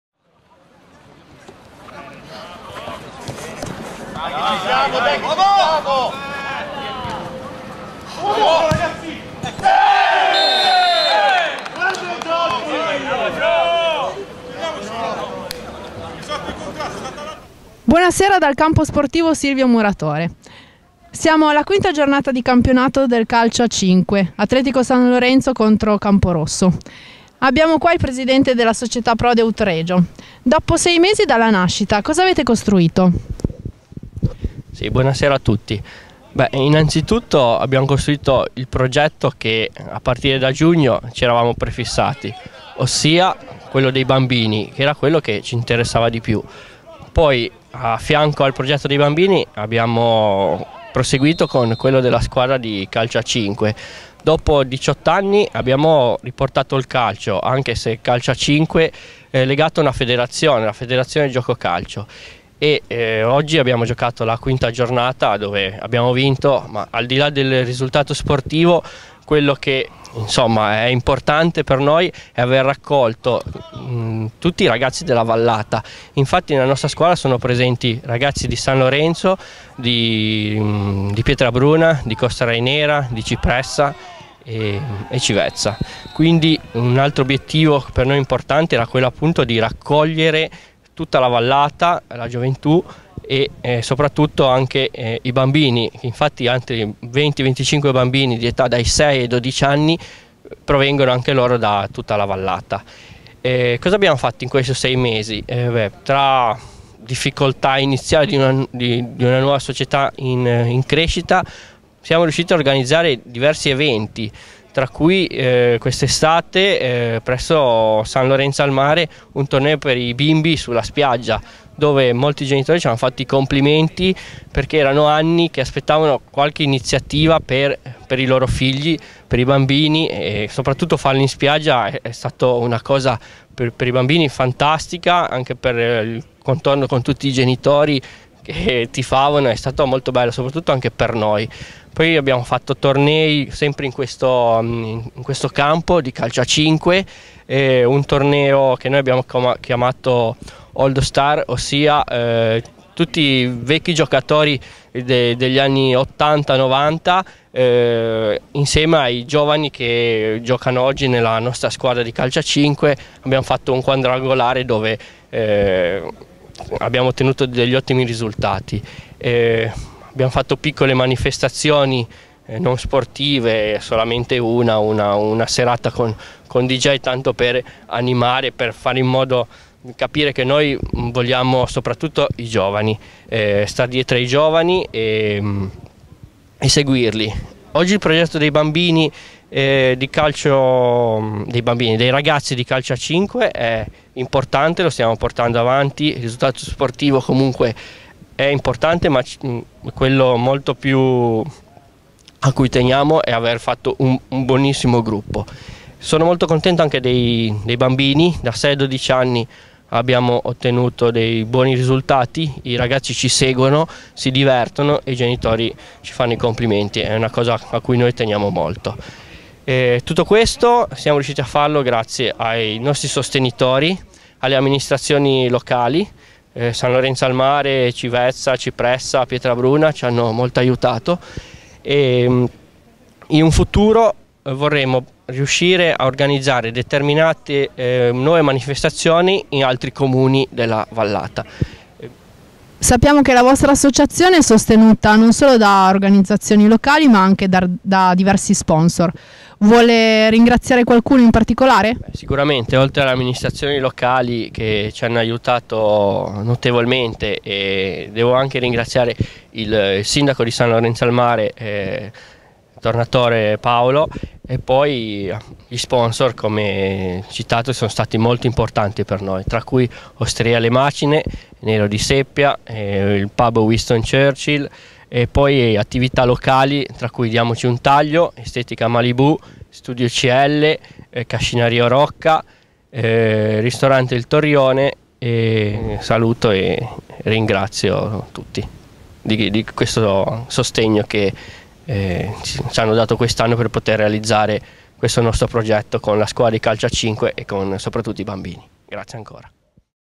Asd Prodeo Ut Regio campionato FICG a 5 serie D
Atletico San Lorenzo - Camporosso 9-1 (4 Littardi, 2 Ghirotti,Cavarero,Gandolfo, Ereddia)

Vittoria netta e meritata per la formazione della Prodeo che raggiunge quota 10 punti in classifica ed è momentaneamente in testa al campionato. Complimenti a tutti i ragazzi e un grazie a tutti i tifosi che sono venuti al campo a sostenere la squadra. Forza Prodeo!